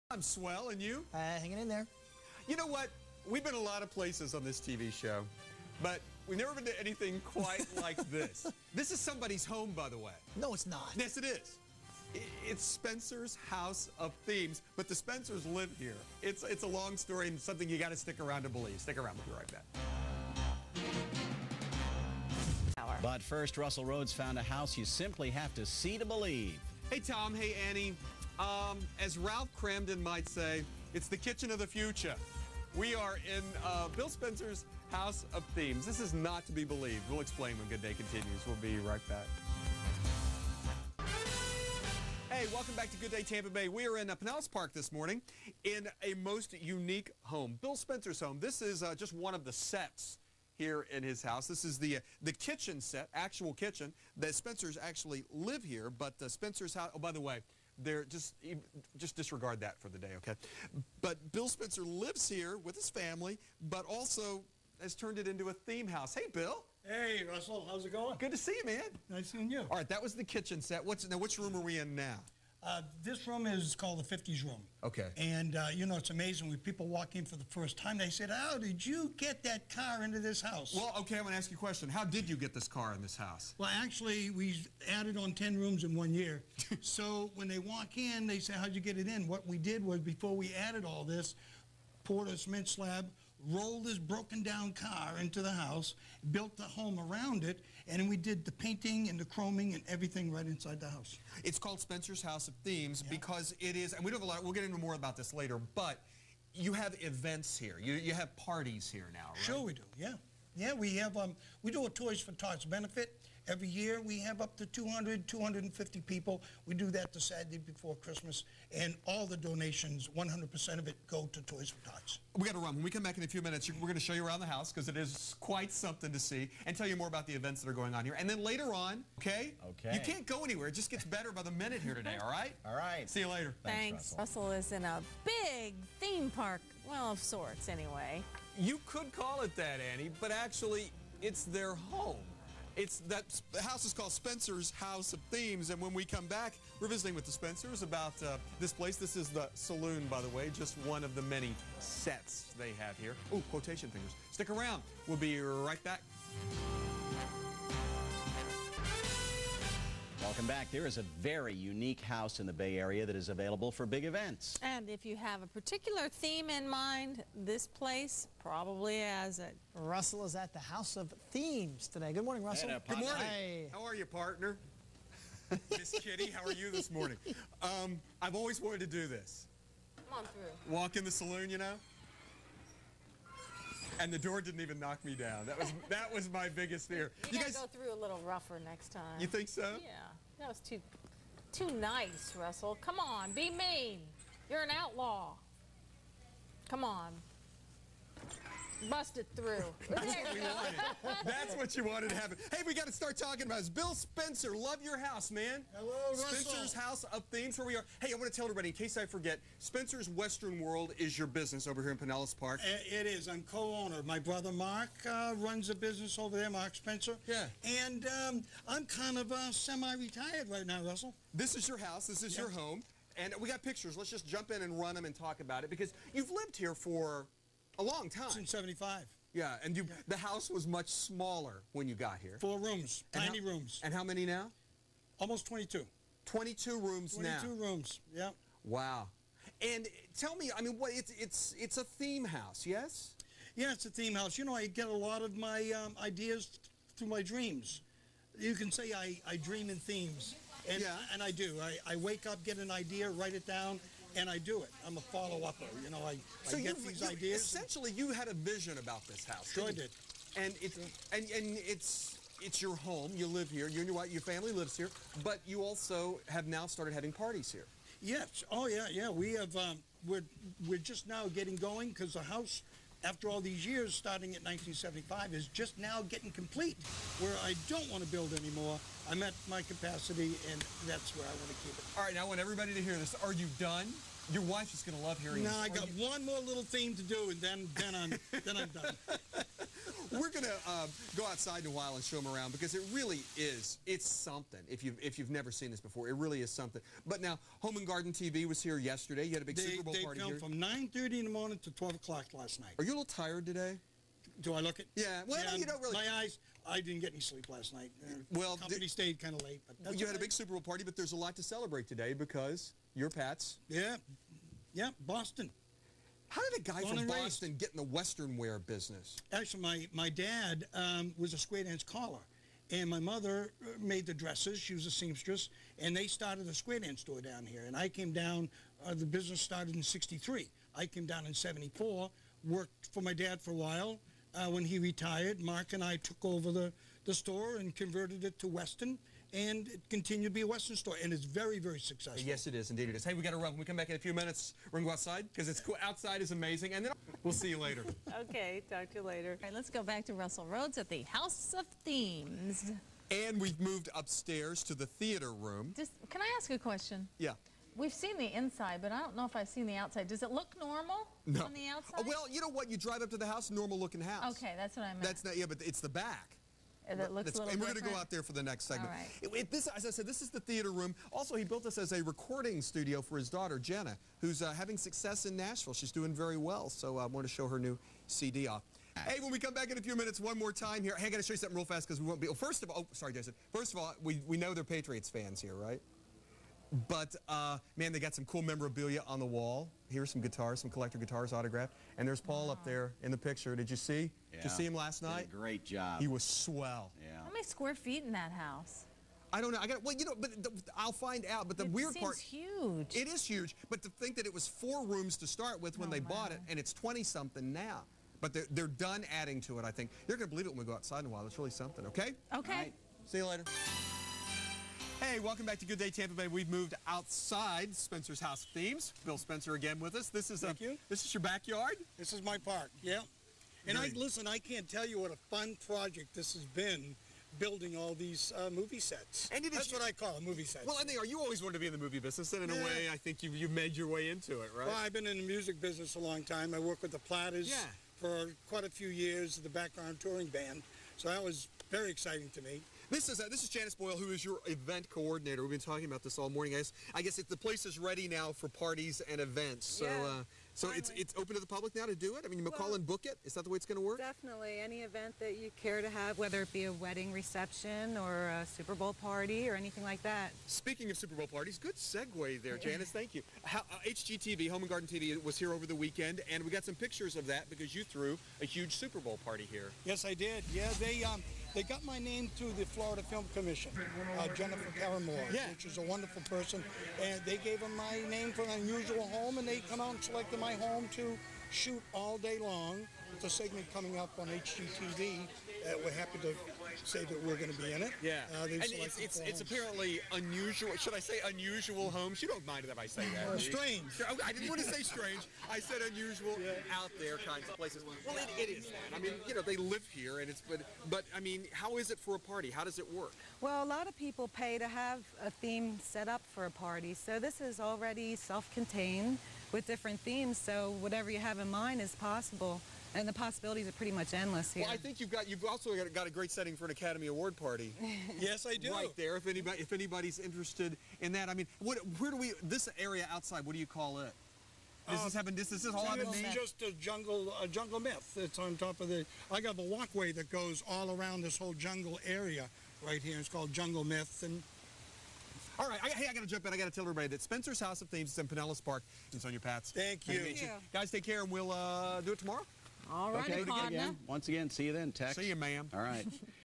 Fox 13, Discovers Bill Spencer's House of Themes
Entertainment house 22 Rooms, Around 6500 square feet. "Features" 30 seat Movie Theater. Full size saloon with balcony, 75 foot pirates cave, Complete with full size 55 Chevy Bell-Air. Bose sound system. Wilderness living room. With double 10' water falls. Taxidermy Lions and 10' kodiak  Bear. Have to see to be-leave. And much more...  Your Complete themed house. As seen TV, and Internationally